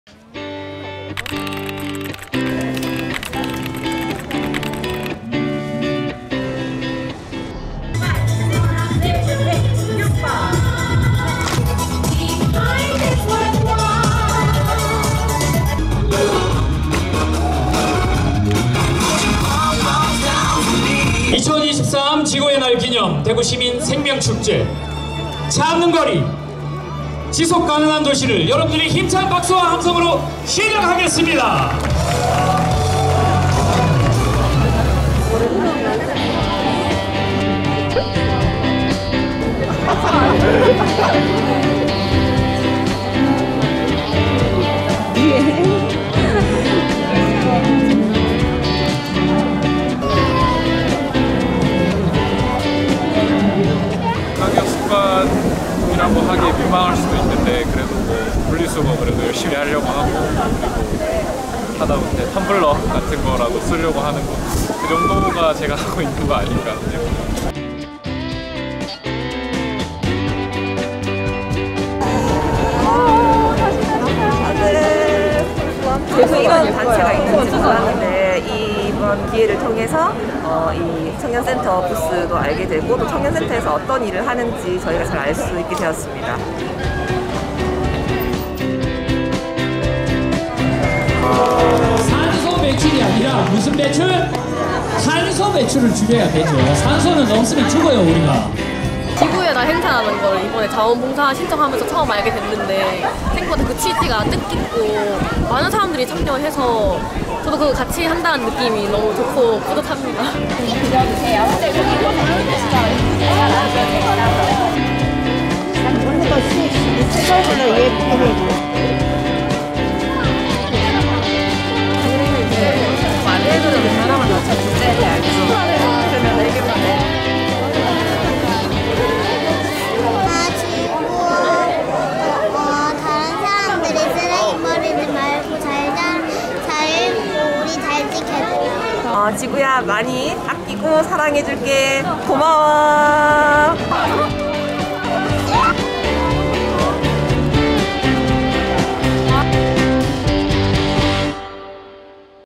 [청년기자단BudDY] 2023지구의날 대구시민생명축제에 가다!
청년기자단 7기가 2023 지구의날을 기념하여 4/22(토) 진행된 대구시민생명축제에 함께 했습니다~!
반월당~중앙로~대구역으로 이어진 24시간 차없는 거리를 가득메운 대구시민들의 즐거운 모습과 청년기자단의 활동을 가볍게 만나보시죠:)